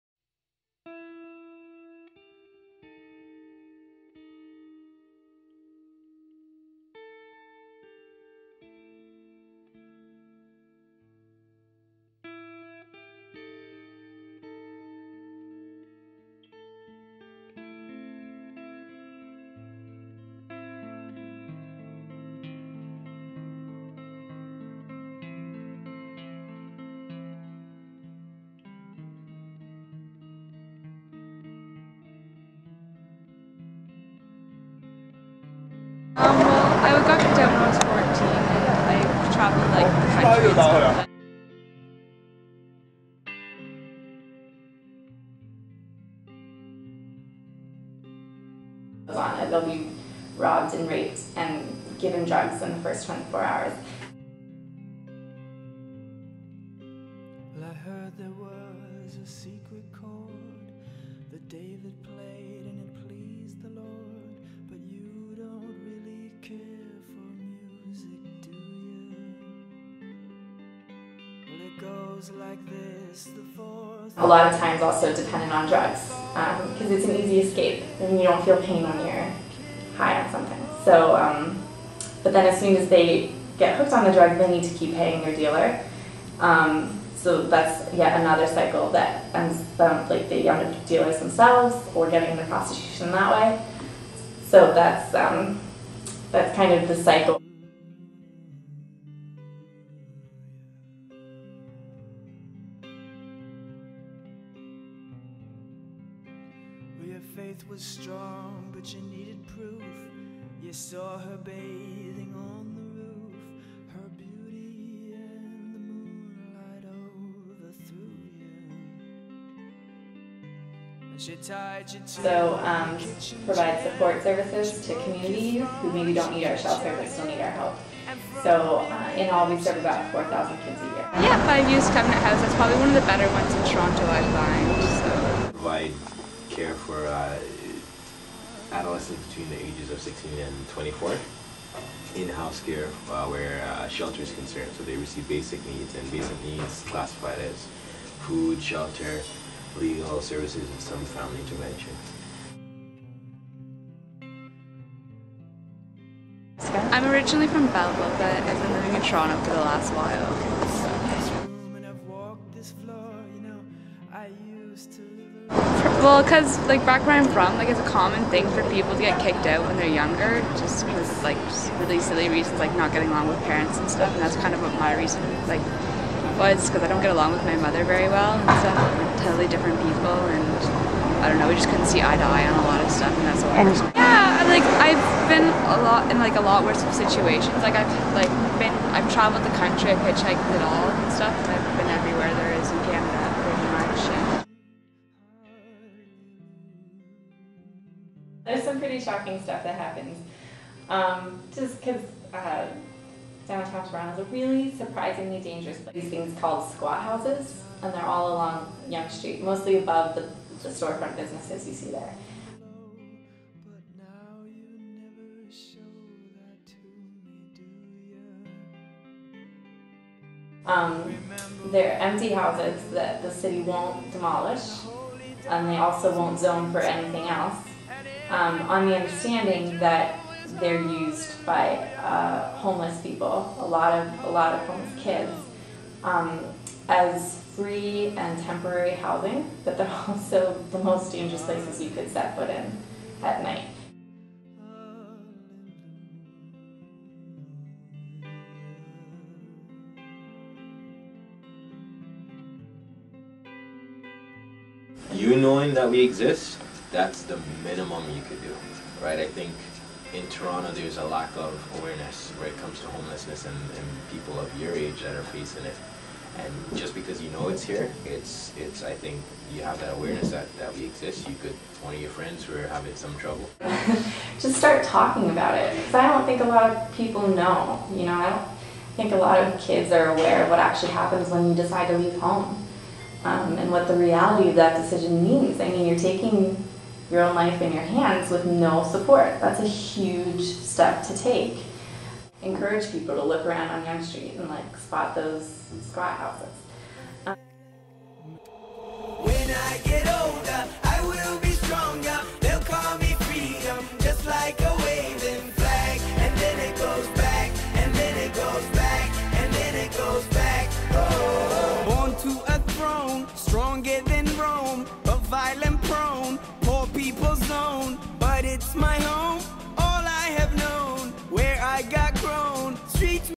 Behind the Faces
This was my grade 10 IB personal project. The pictures are either taken by me or taken from various sources. The interviews are with Cathy Checkly who works at Covenant House in Toronto and Anthony Stewart from Touchstone Shelter.
This was made to raise awareness about homeless youth and encourage people to help this issue in their community.